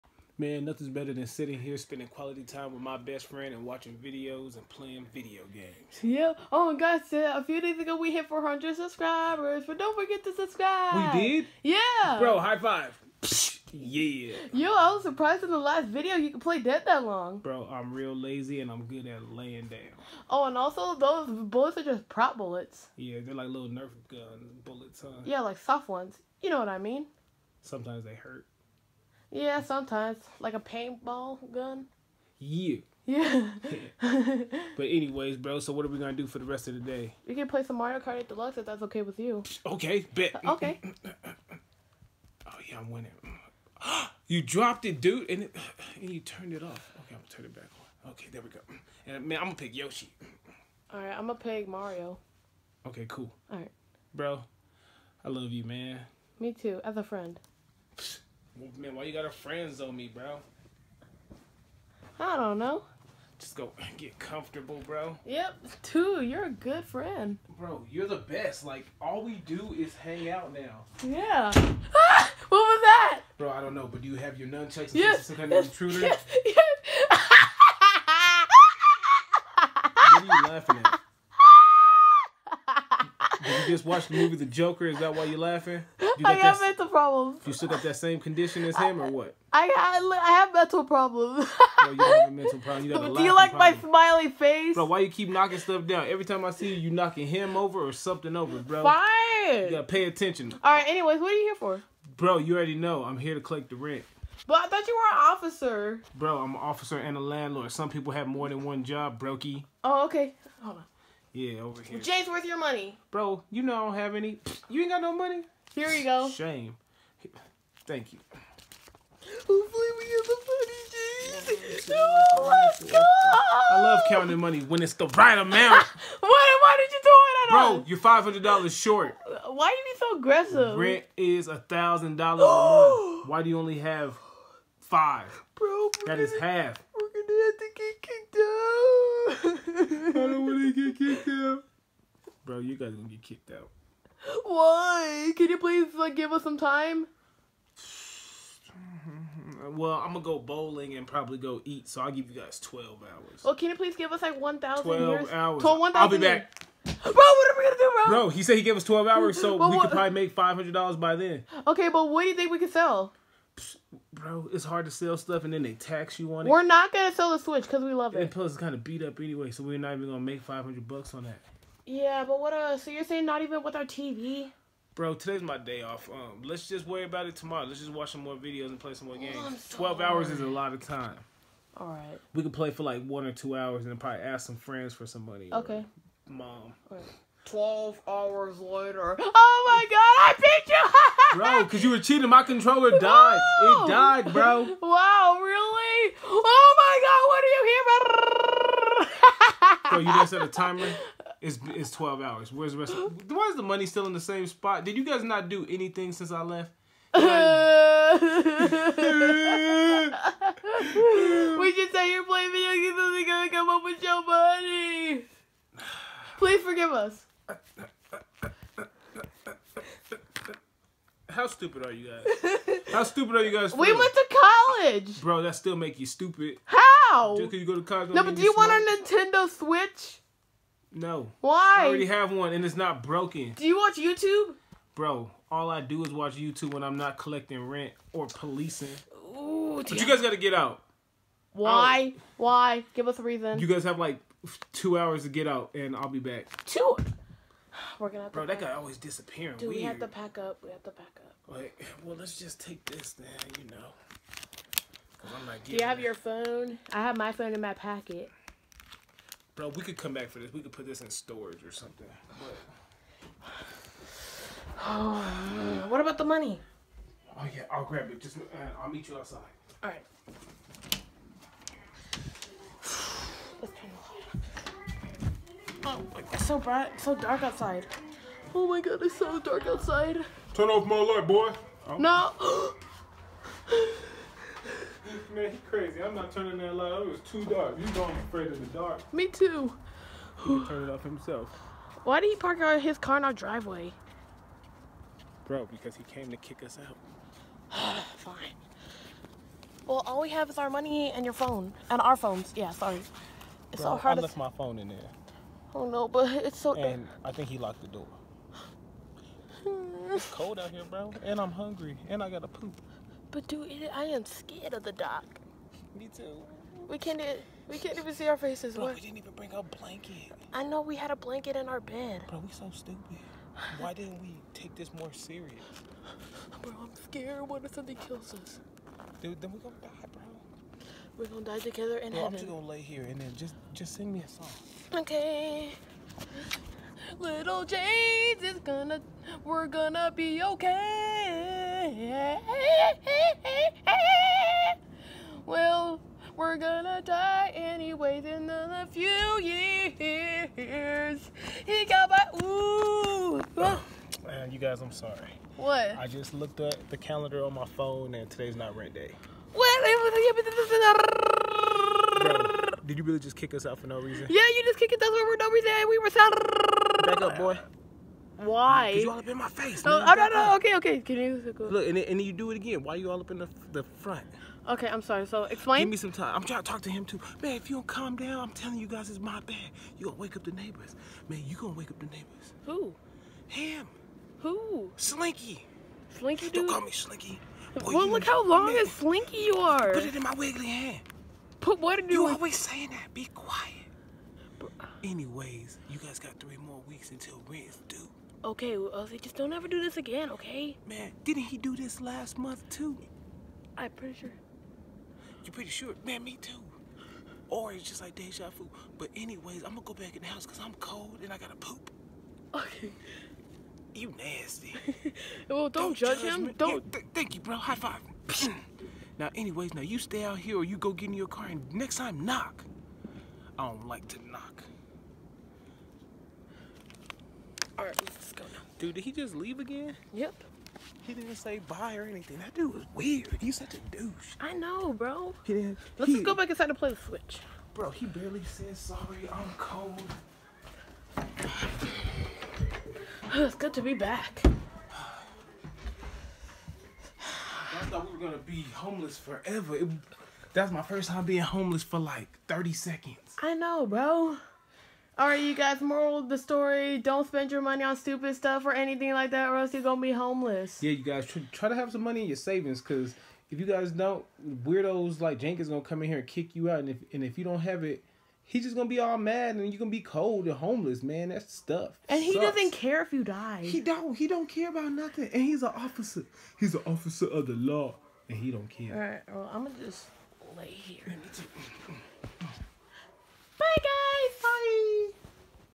CTA Movie: The Brokies
Ted and James didn’t had the money for rent.